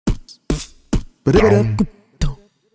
19.12 : rotoscope?
testing the little equipment and software i have to see what a simple rudimentary rotoscope wpuld look like. i enjoy wathcing this one haha